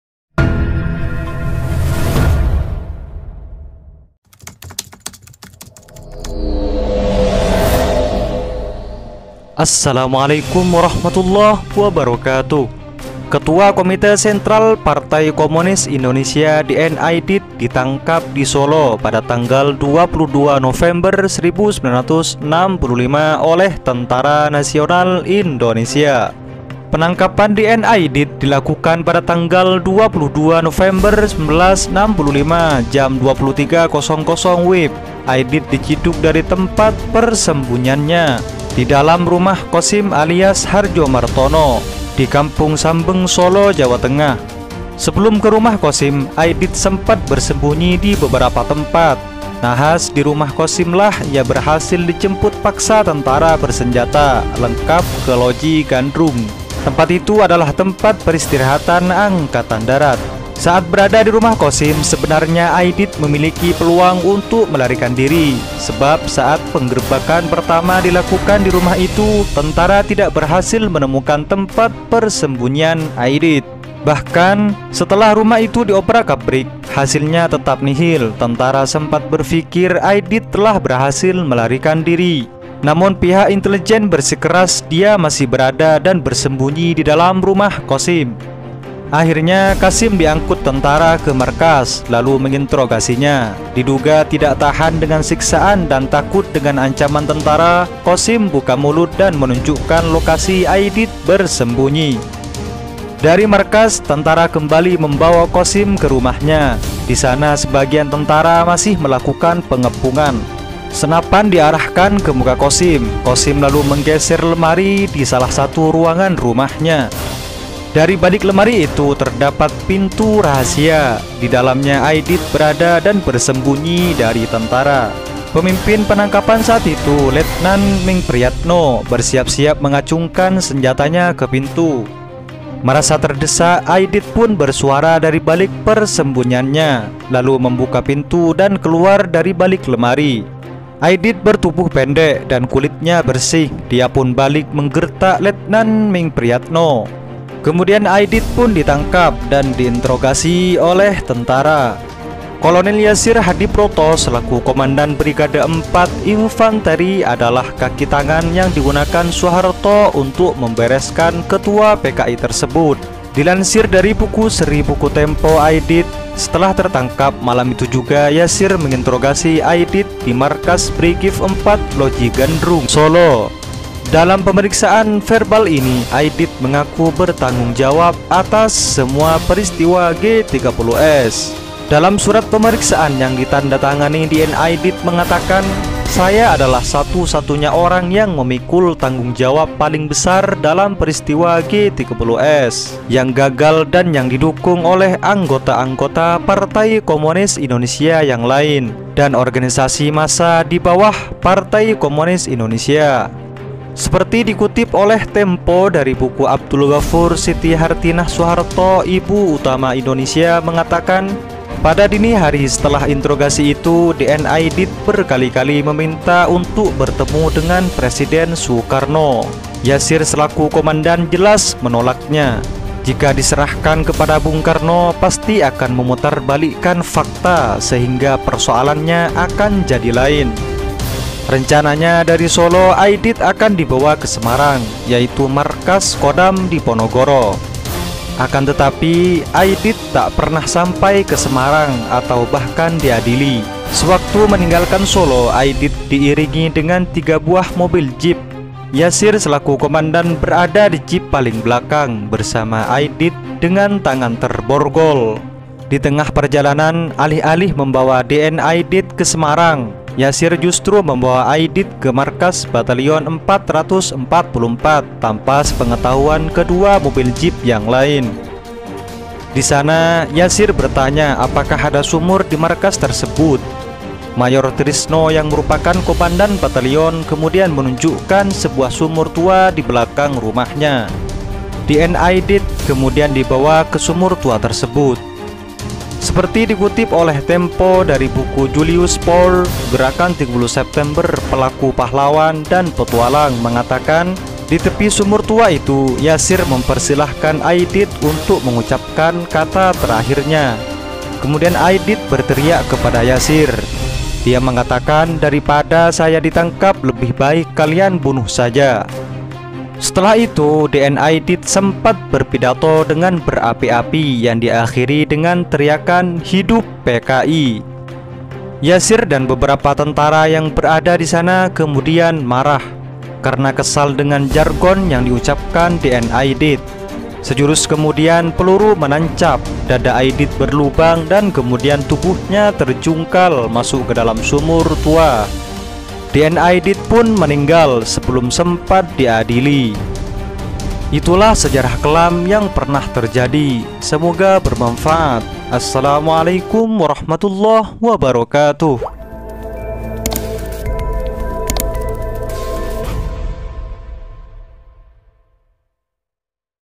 sejarah t3rbuπuhny∆ ketua PKI karena ucap dua kalimat ini...!!!
Pidato T3r4khir DN Aidit di Sumur Tua Markas Batalyon.

Ketua Committee Central Partai Komunis Indonesia DN Aidit dit4n9k4p di Solo pada 22 November 1965 oleh Tentara Nasional Indonesia. Kolonel Yasir Hadibroto selaku Komandan Brigade IV Infanteri adalah kaki tangan yang digunakan Soeharto untuk “memb3r3skan” Aidit.

Sumber : Buku “Seri Buku Tempo: Aidit”, Buku Abdul Gafur “Siti Hartinah Soeharto: Ibu Utama Indonesia”, Buku Julius Pour “Gerakan 30 September: Pelaku, Pahlawan & Petualang”. Tirto.id

Semoga Bermanfaat

Doakan..semoga tercapai🙏🙏